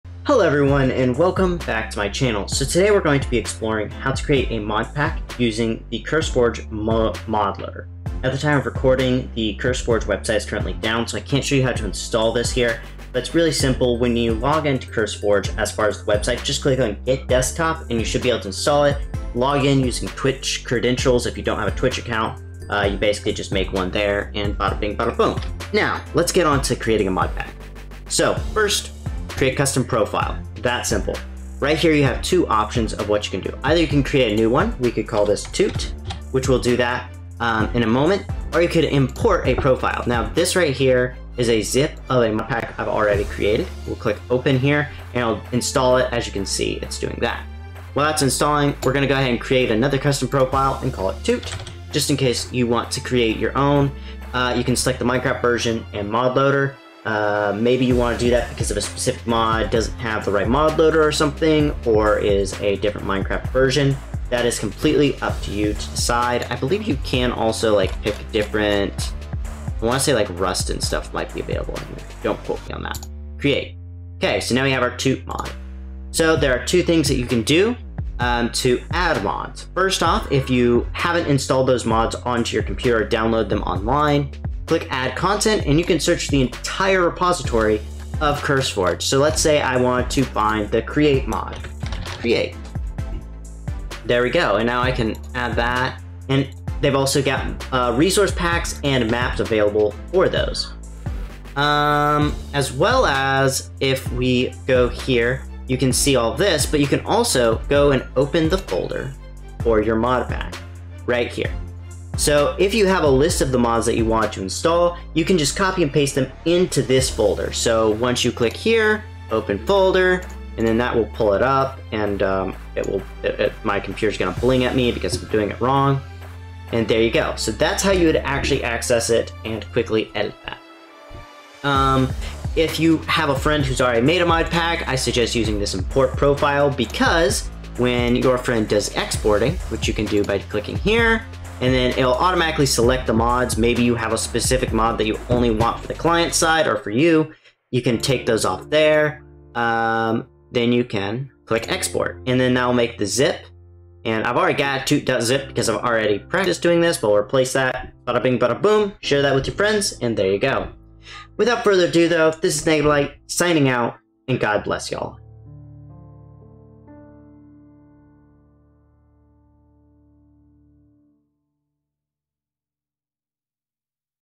Make Custom Modpack with Curse Forge
Ever want to make your own mod packs. This is how.